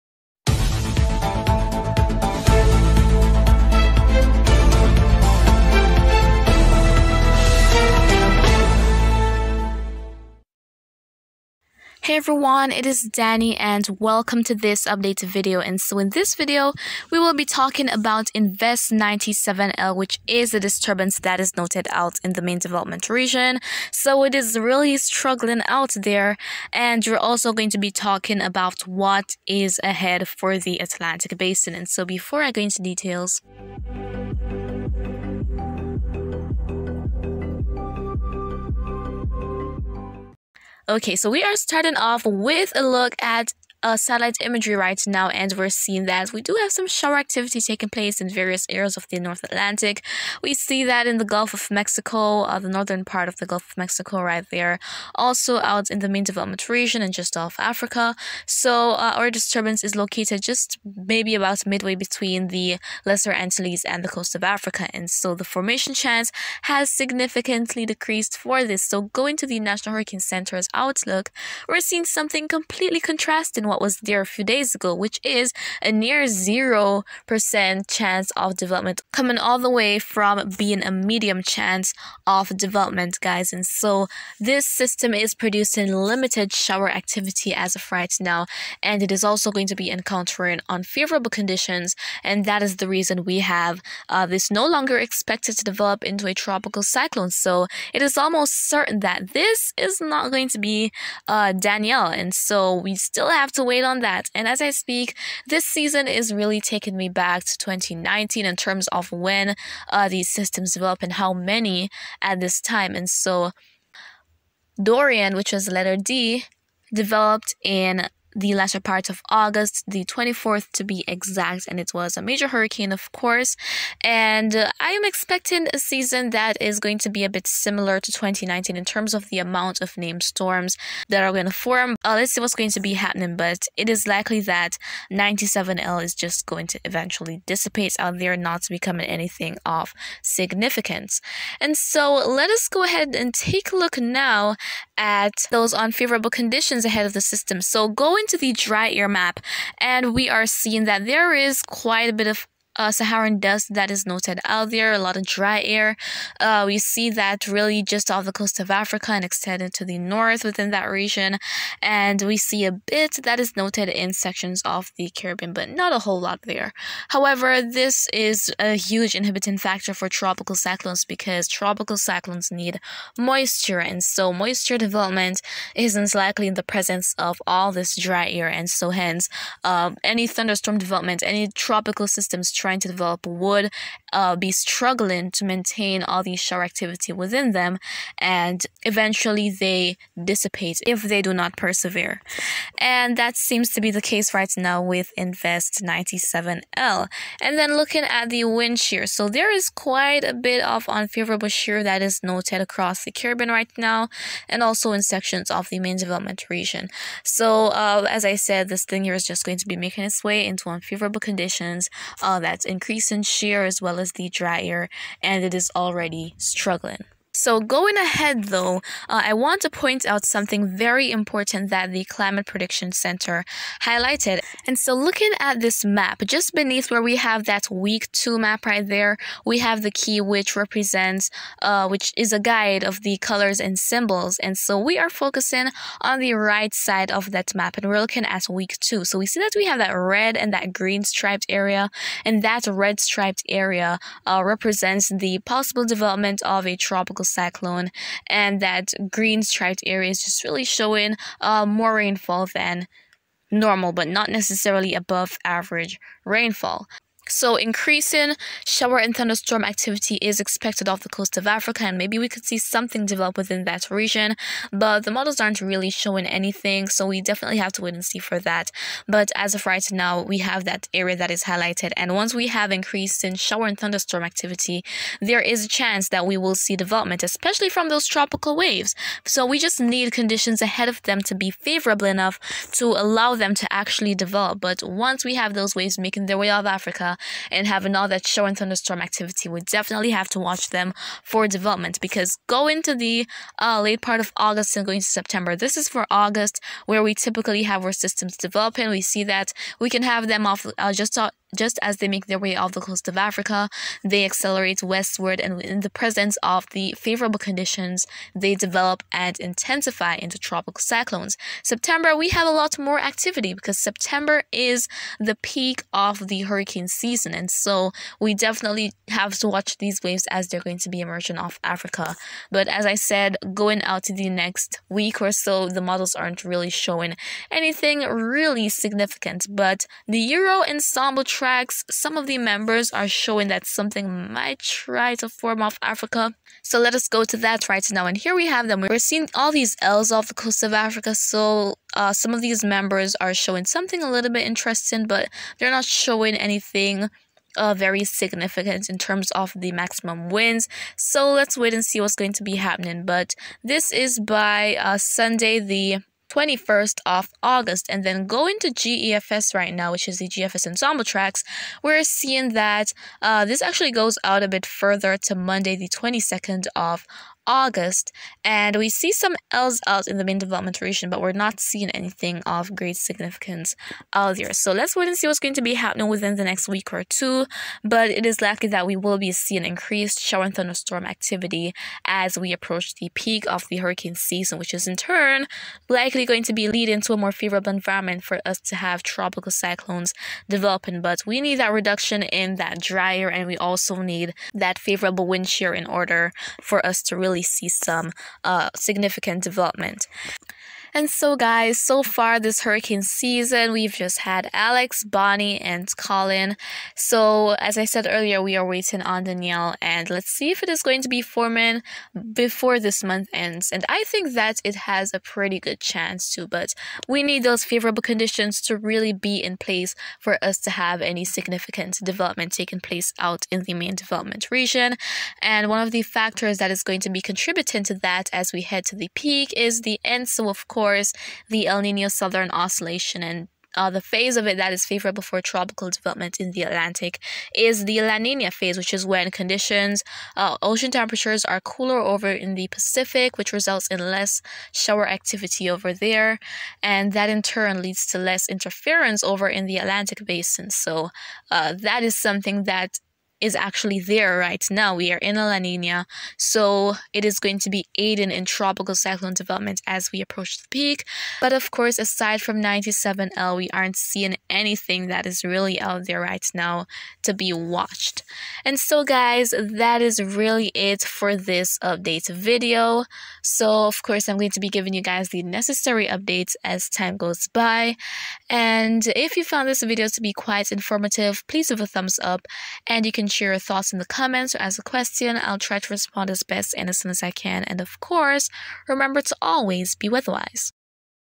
Tropical disturbance heads westward as formation chance decreases, more activity ahead
~weather.gurl.danii